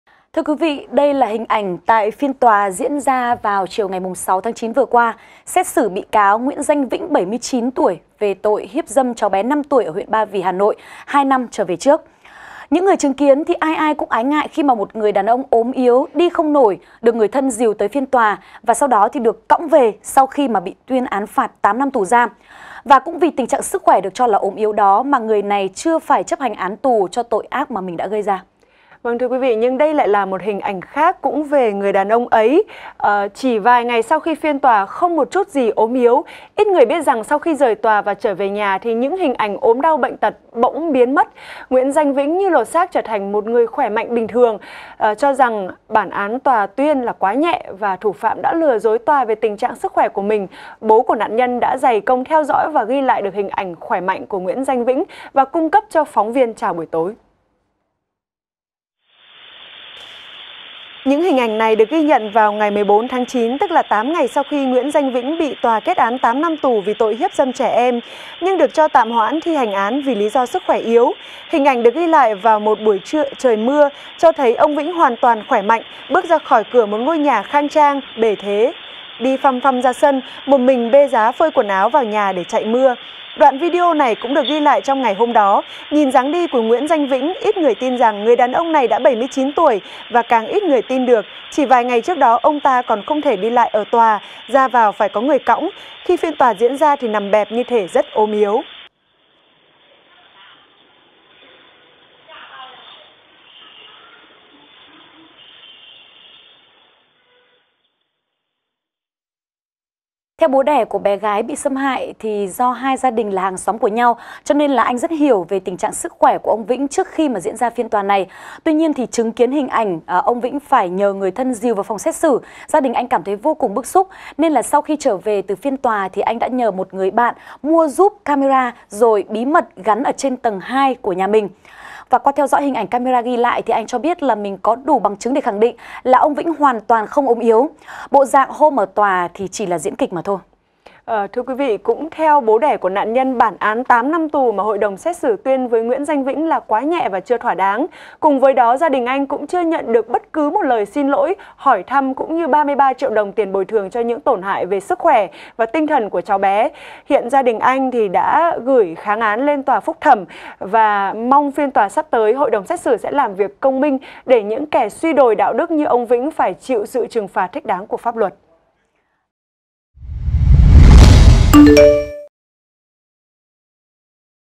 VTC14 | Vụ ông già 79 hiếp dâm trẻ em: Lộ bằng chứng phạm nhân lừa tòa
(VTC14) - Ít người biết rằng/ sau khi rời tòa và trở về nhà, thì những hình ảnh ốm đau bệnh tật bỗng biến mất, Nguyễn Danh Vĩnh như lột xác trở thành một người khỏe mạnh bình thường.